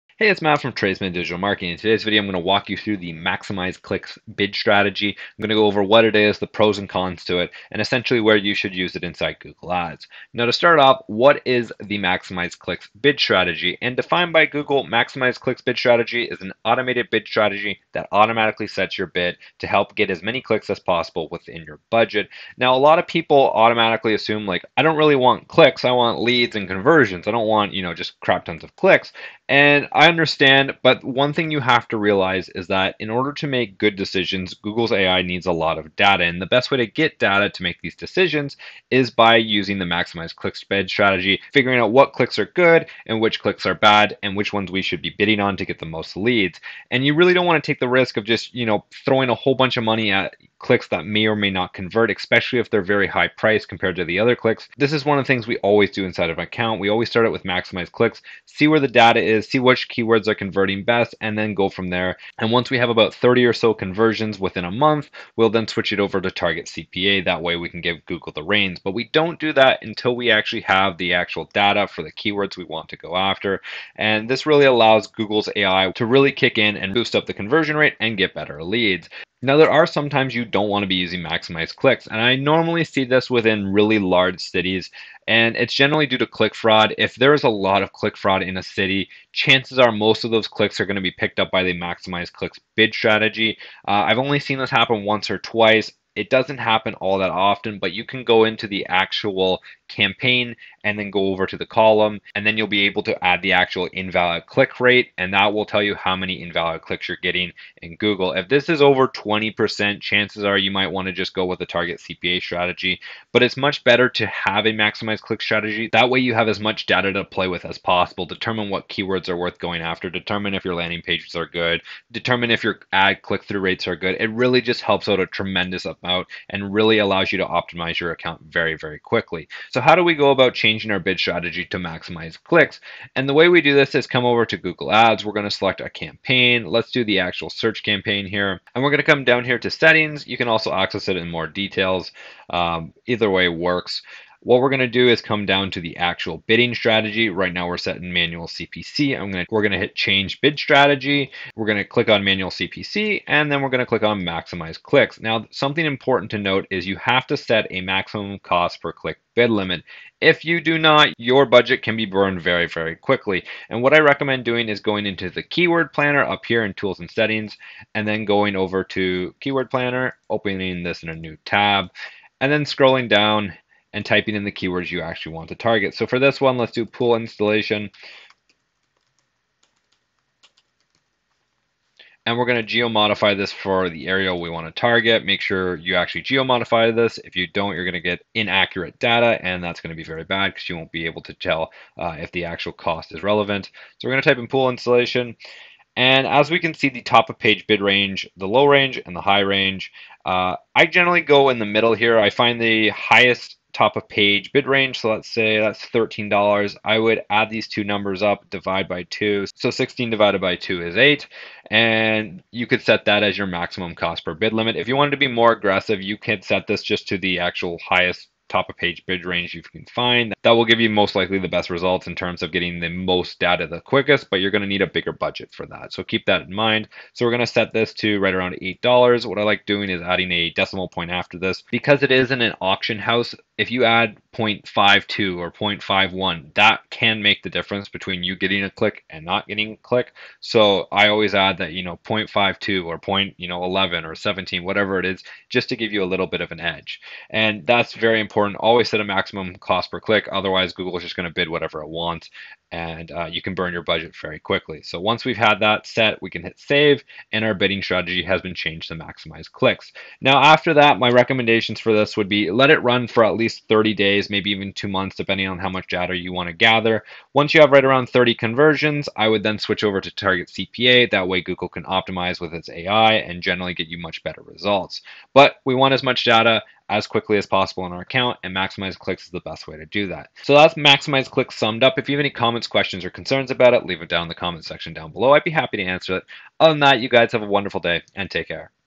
Maximize Clicks (2022) - How To Use Maximize Clicks Bidding Strategy In Google Ads
In this video, on maximize clicks l will explain How To Use Maximize Clicks Bidding Strategy In Google Ads I will also go over when not to use the maximize clicks bidding strategy in 2022.

0:00 Intro
0:10 What Is Maximize Clicks Bid Strategy?
0:20 Why You Should Use It
1:19 When You Shouldn't Use It
2:09 How To Set Your Account To Maximize Clicks
2:34 Maximum Bid Limit (Important)
4:20 My Recommendations

Maximize clicks: Definition

An automated bid strategy that automatically sets your bids to help get as many clicks as possible within your budget.

Maximize Clicks is the simplest way to bid for clicks—you set a budget, and Google Ads does the rest. You don't need to choose specific bid amounts for your ad groups, keywords, or placements.

With Maximize Clicks, you set an average daily budget and the Google Ads system automatically sets your maximum cost-per-click (CPC) bids on your behalf, with the goal of getting you the most clicks possible within that budget.

For more control, you can also set bid limits to prevent your bids from being set over that amount. While setting a bid limit offers tighter control, it might restrict your ad position and the number of clicks that your ad receives.

If at any time you don't like the bid suggestions, you can easily switch to using Manual CPC bidding, which gives you full control to set the bid amounts yourself.

Maximize Clicks isn't available with the following:

Display Network campaigns with the marketing objectives "Engage with content," or App campaigns.
Bid adjustments based on day and time

Maximize Clicks was previously referred to as “Automatic bidding”

Maximize Clicks is a great bid strategy to gather data inside google ads and is the bidding strategy we recommend starting most google ads campaigns with.